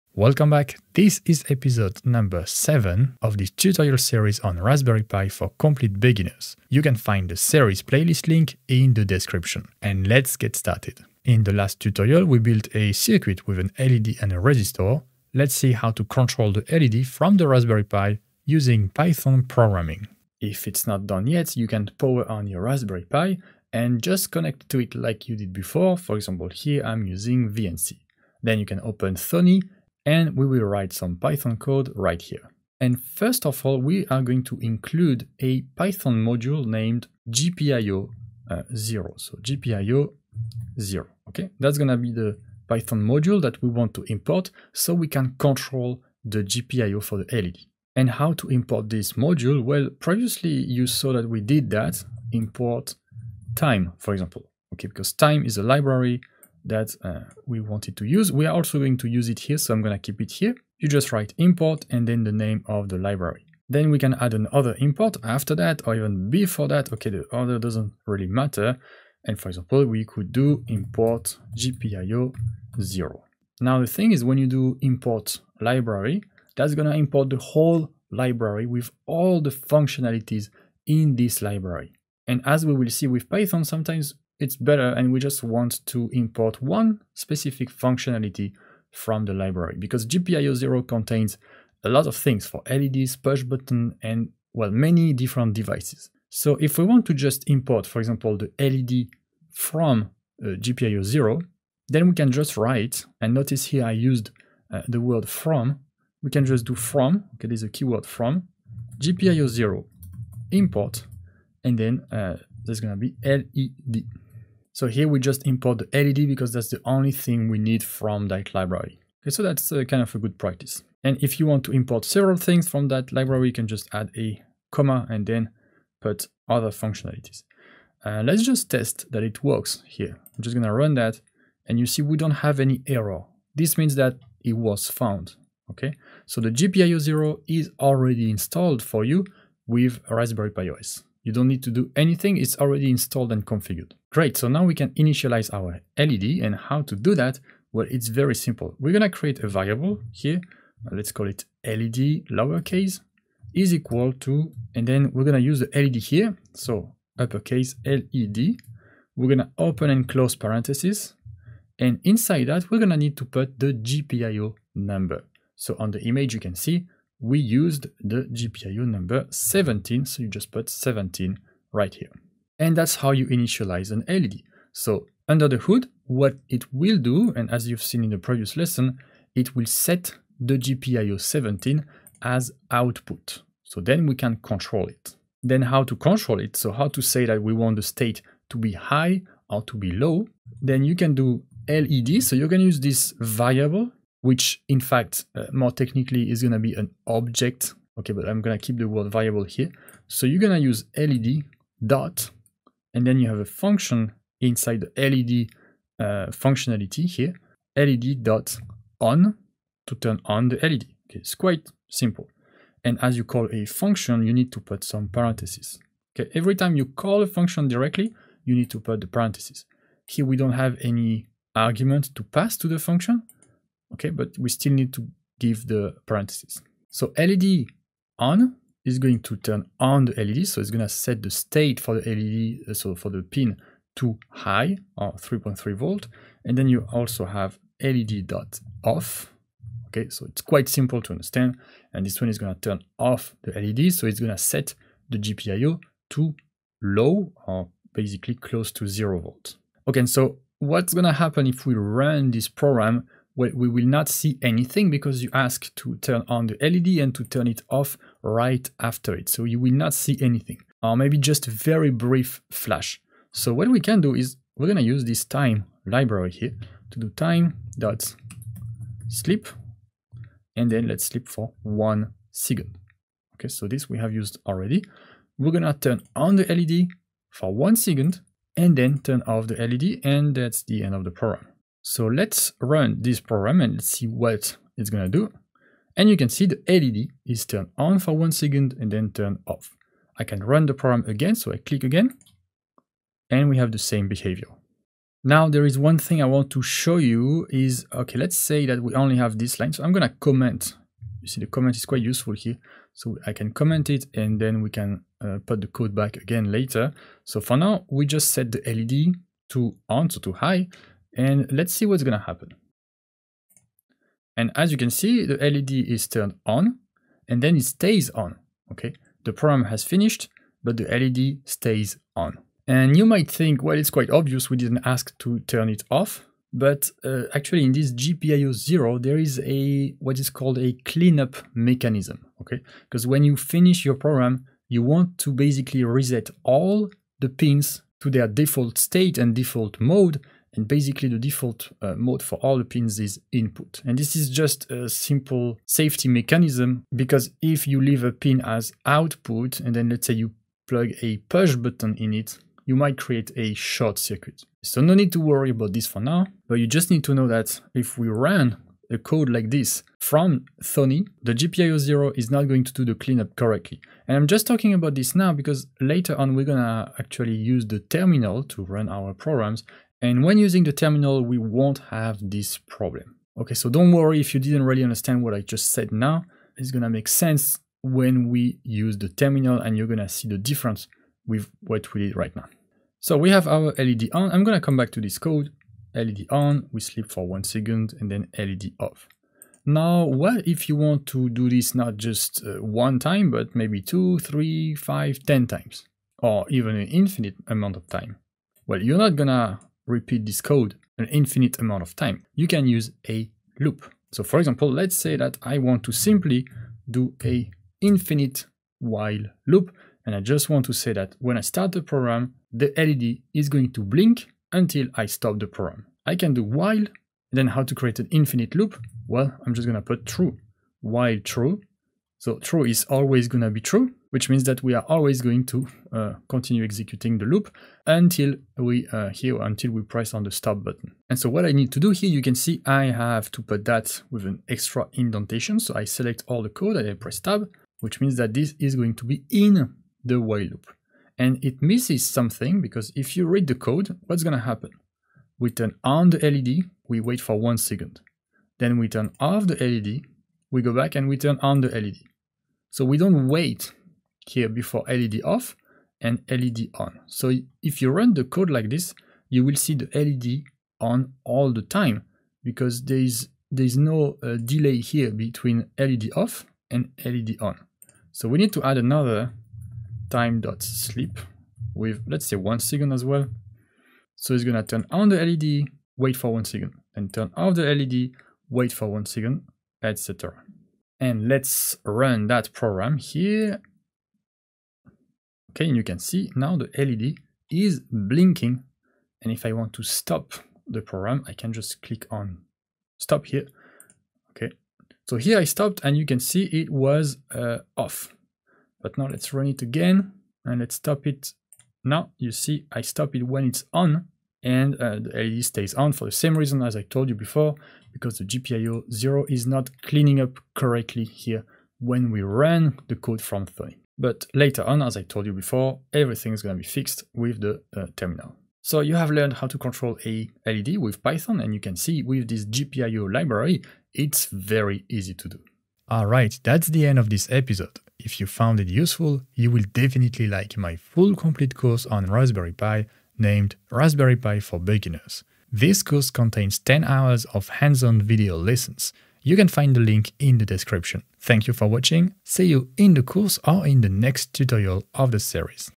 Make an LED Blink with Python - Raspberry Pi 5 Tutorial (#7)
In this Raspberry Pi tutorial you will use the circuit you’ve built in a previous tutorial to make an LED blink with Python code, using the gpiozero library.

⏱️ Chapters
(00:00) Initialize the LED with gpiozero
(03:22) Make the LED blink once
(06:29) Make the LED blink indefinitely